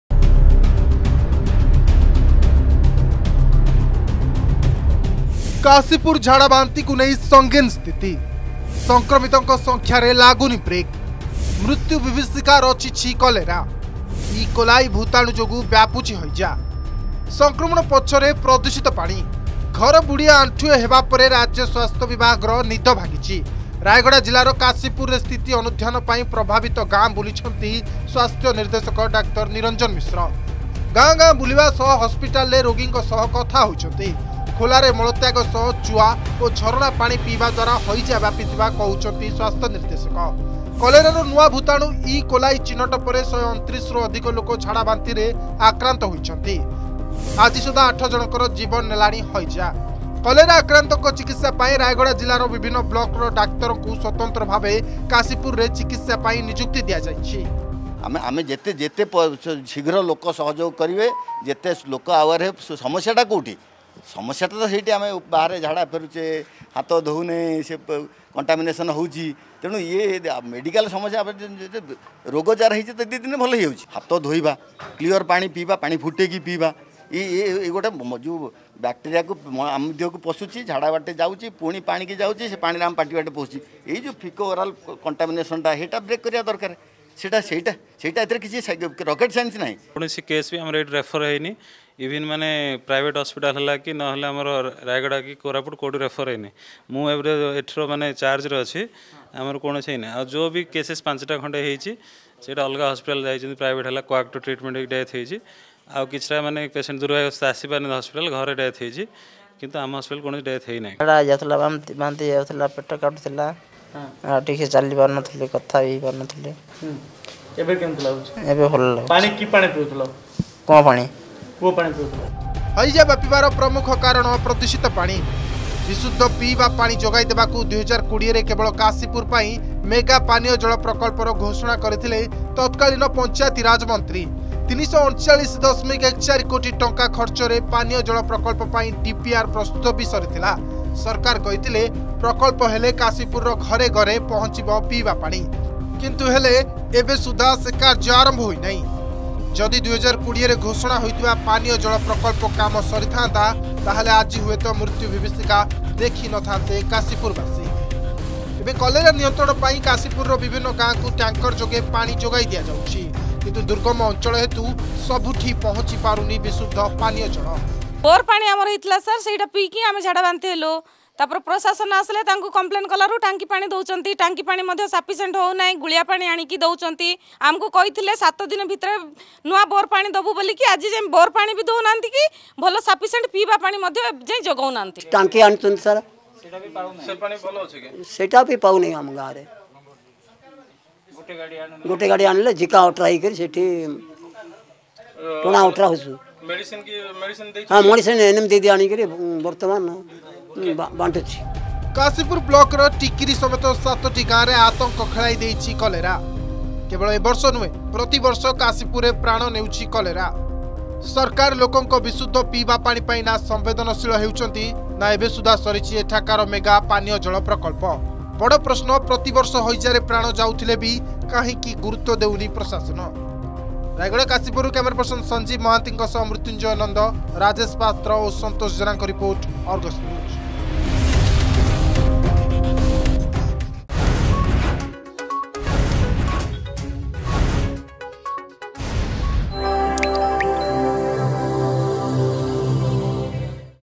ରାୟଗଡା କାଶୀପୁରରେ କଲେରାର ସାଂଘାତିକ ସ୍ଥିତି
ଘର ବୁଡି ପାଣି ଆଣ୍ଠୁଏ ହେବା ପରେ ତତ୍ପରତା ଦେଖାଇଛି ସ୍ବାସ୍ଥ୍ୟ ବିଭାଗ। ରାୟଗଡା ଜିଲ୍ଲାର କାଶୀପୁରରେ ସ୍ଥିତି ଅନୁଧ୍ୟାନ ପାଇଁ ପ୍ରଭାବିତ ଗାଁ ବୁଲିଛନ୍ତି ସ୍ବାସ୍ଥ୍ୟ ନିର୍ଦେଶକ ଡାକ୍ତର ନିରଞ୍ଜନ ମିଶ୍ର